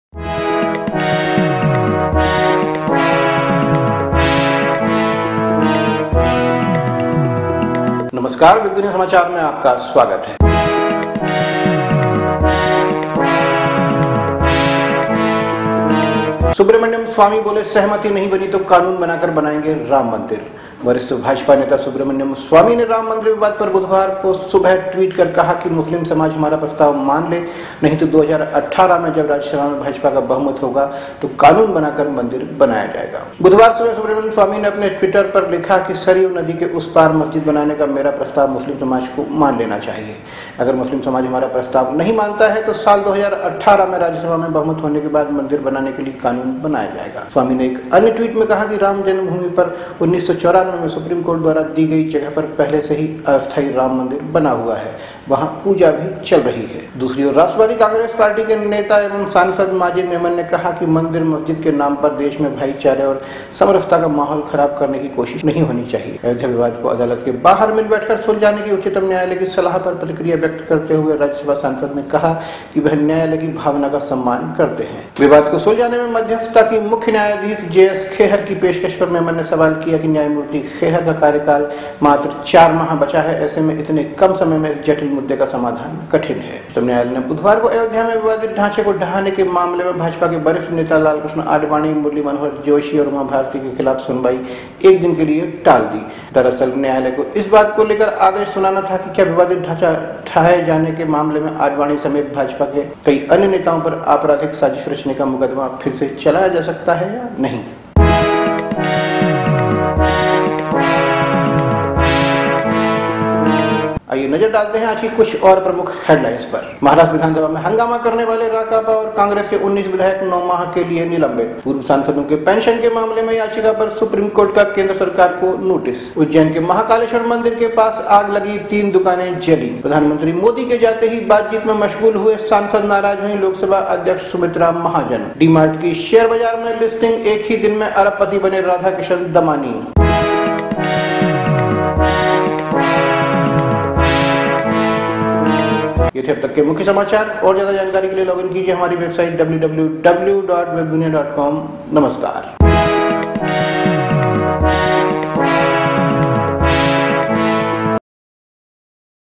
सहमति नहीं बनी तो कानून बनाकर बनाएंगे राम मंदिर - सुब्रमण्यम स्वामी | Ram Mandir Temple Ayodhya
वरिष्ठ भाजपा नेता सुब्रमण्यम स्वामी ने राम मंदिर विवाद पर बुधवार सुबह ट्वीट कर कहा कि मुस्लिम समाज हमारा प्रस्ताव मान ले नहीं तो 2018 में जब राज्यसभा में भाजपा का बहुमत होगा तो कानून बनाकर मंदिर बनाया जाएगा।